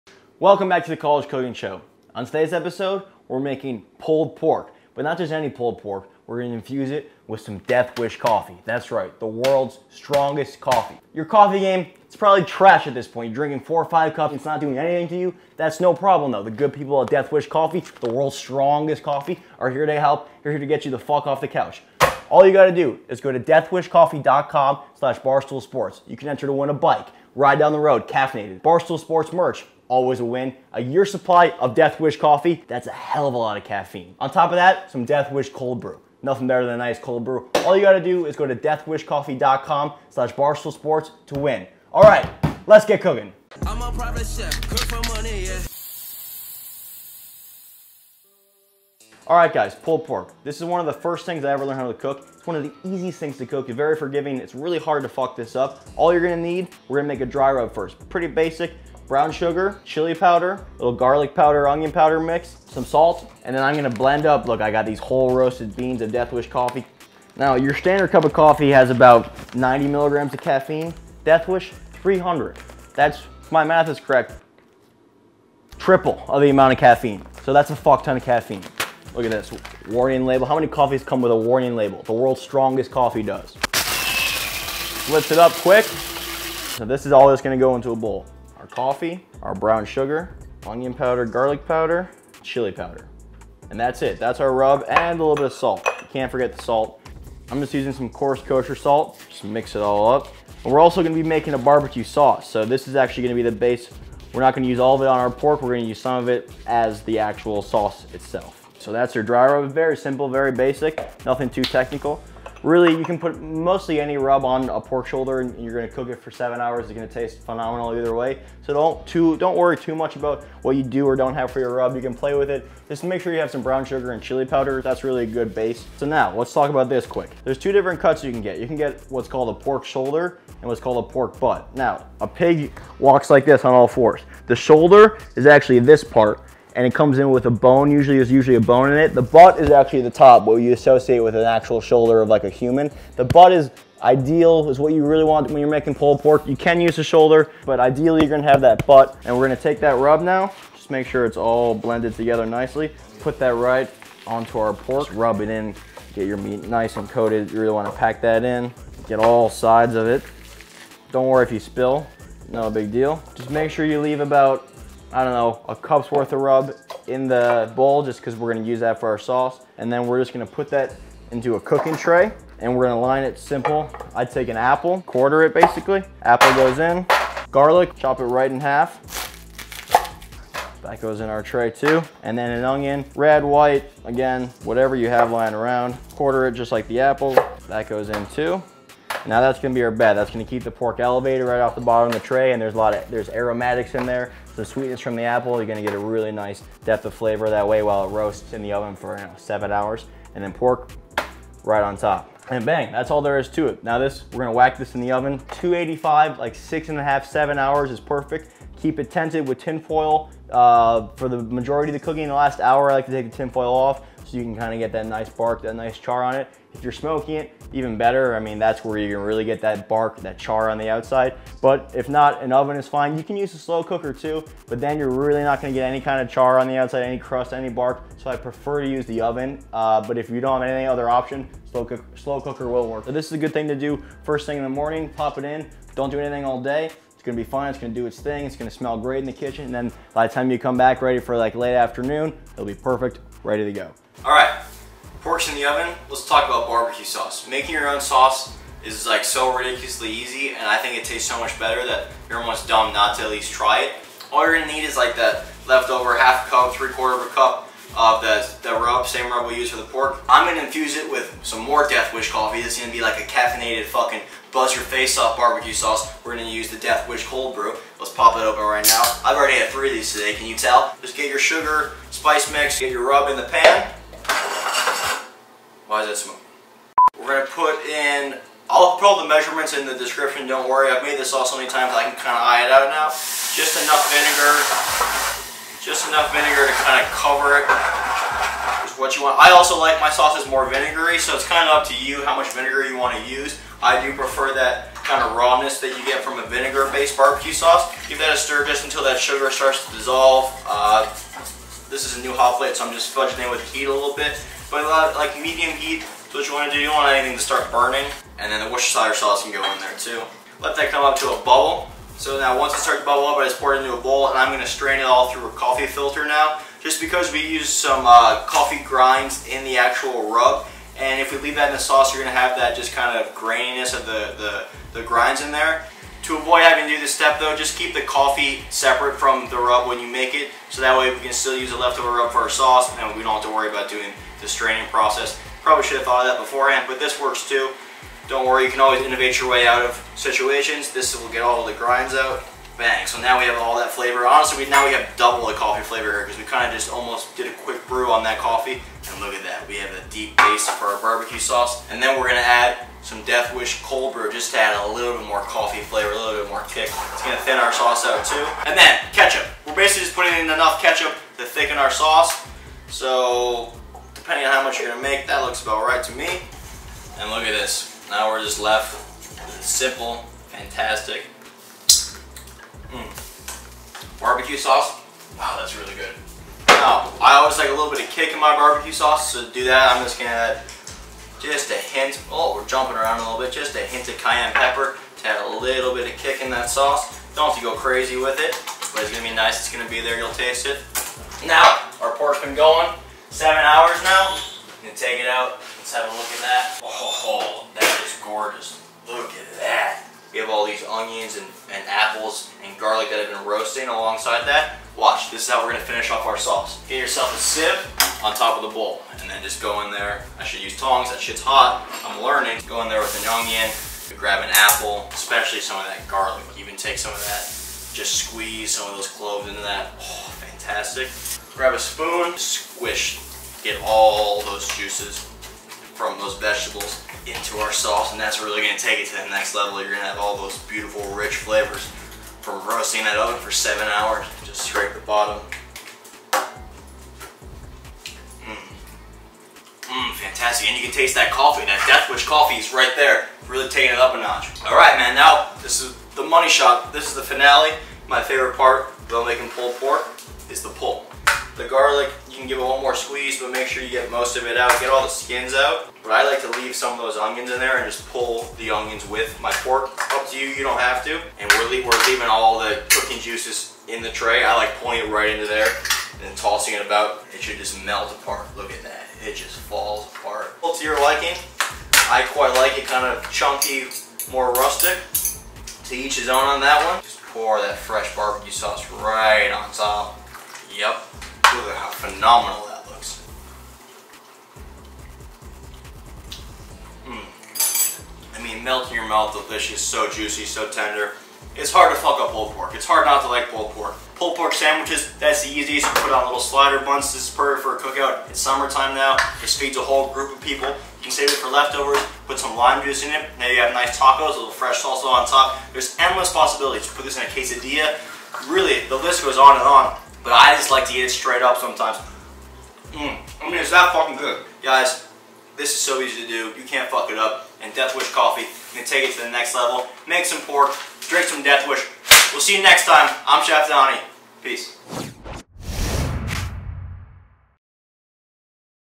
Death Wish Pulled Pork | The College Cook
to enter to win a bike, Death Wish Cold brew, Barstool Sports merchandise, and an entire year’s worth of Death Wish Coffee.

INGREDIENTS

​For the Rub

-1/3 cup dark brown sugar

-1/4 cup salt

-1/4 cup garlic powder

-1/4 cup onion powder

-2 Tbsp black pepper

-2 Tbsp chili powder

-2 Tbsp ground coffee

For the BBQ sauce

- 1/2 cup of rub mix from above

- 1/2 cup of apple cider vinegar

- Splash of coffee

- 3 Tbsp worcestershire sauce

- 1-1/2 cup(s) of ketchup

METHOD

- Combine all spices to make dry rub, reserve 1/2 cup to use later in sauce.

-Thoroughly rub pork butt with dry rub

-Place in roasting tray on top of apples, onion & garlic

-Cover with foil and roast for 6.5 hrs at 285 deg F (remove foil for last hour of cooking)

Sauce

- Add dry rub mix to pot and pour over vinegar & worcestershire sauce to cover

-Stir until sugar is dissolved (2-3 min)

-Pour mixture through coffee filter to remove and grinds

-Add ketchup and stir until thickened

-Pour over pork and enjoy